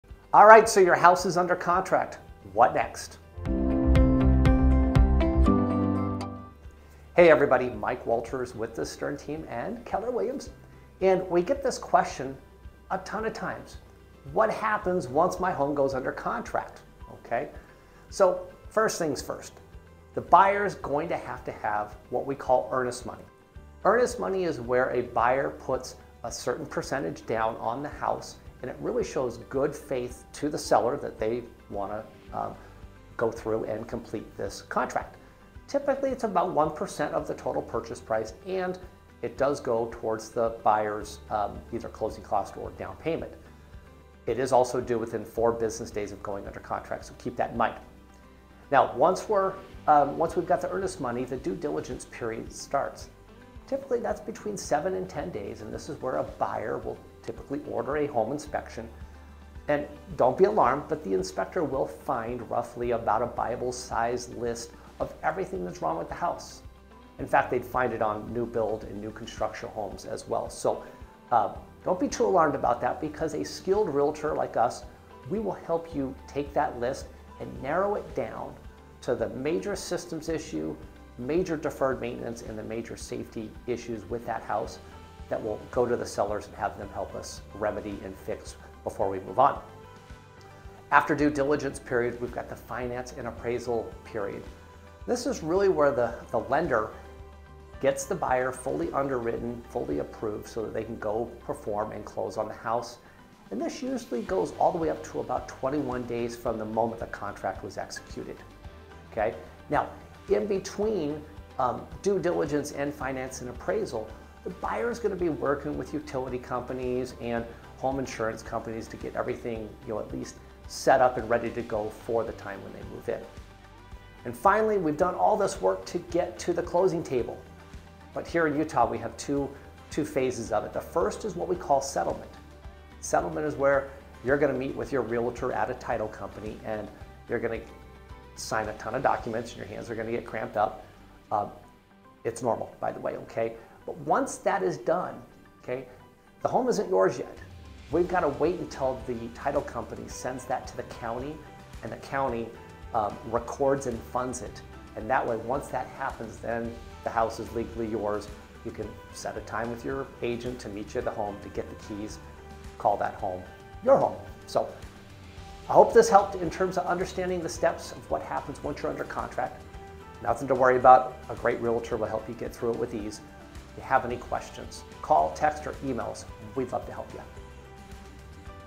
Stern Market Minute: You're Under Contract, Now What?
Welcome to today's Stern Market Minute! Are you a beginner regarding being under contract? Feeling lost, confused, or scared but still curious to know everything? No worries, you're in the right place! Join us today as we take you on this amazing informative journey to answer all the burning questions you have, regarding being under contract. We'll explore all the areas in depth, helping you feel confident in your real estate transaction journey! Our professional and experienced guests will be sharing some valuable insights about being under contract that will help to get you excited. By the end, you will have clear answers to all your questions, and we're confident that this will boost your confidence to the next level! Get ready to join us today!